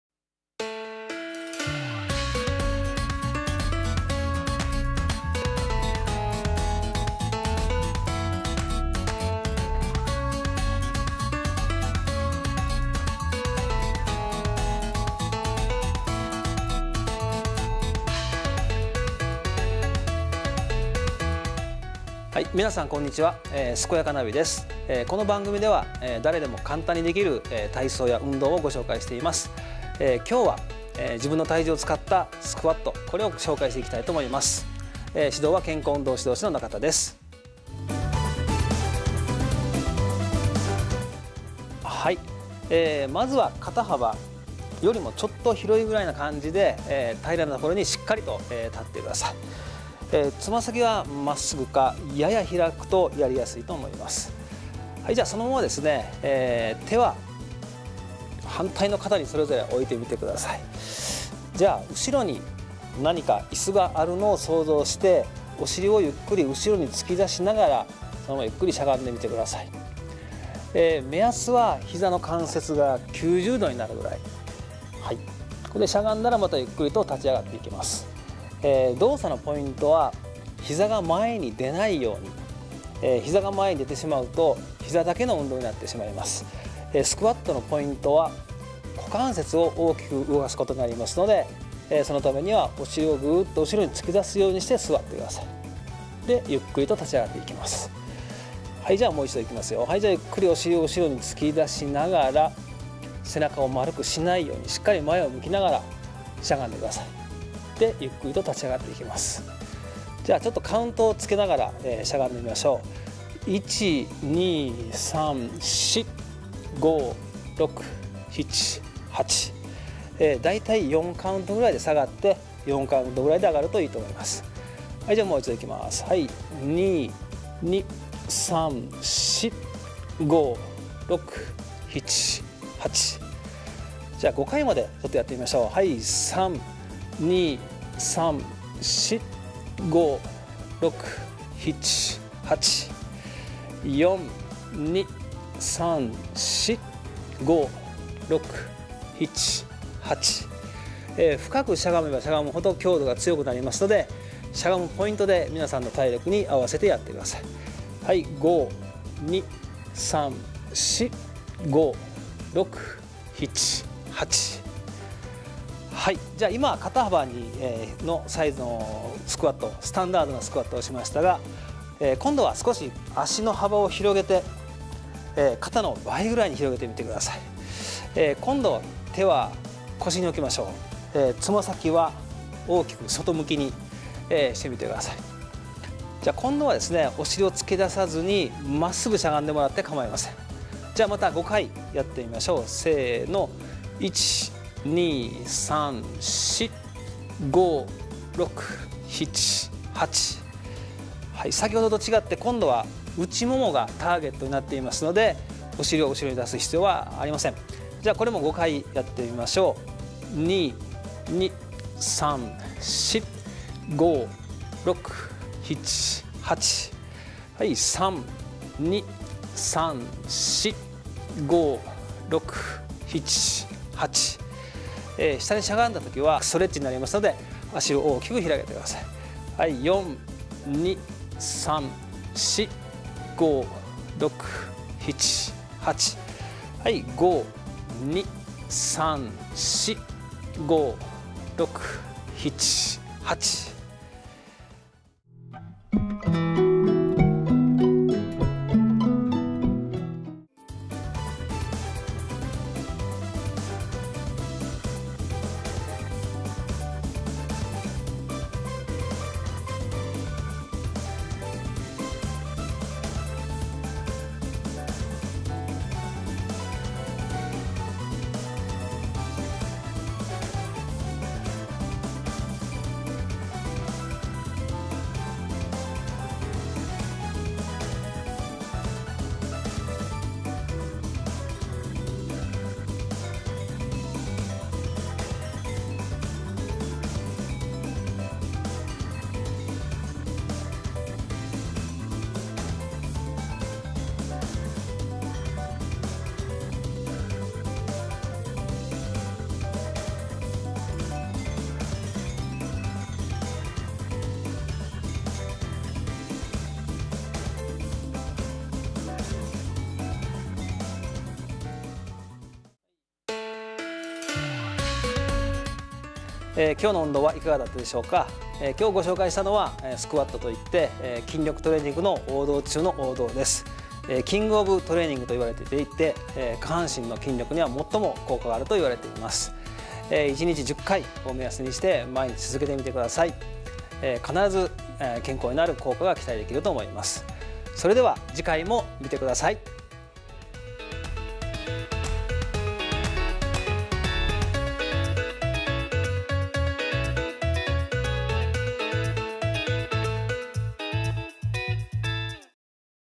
すこやかナビ【自分の体重を使ったスクワット】
健康運動指導士の中田裕幸さんが簡単なストレッチを紹介するすこやかナビ。今回は『自分の体重を使ったスクワット』をお送りします。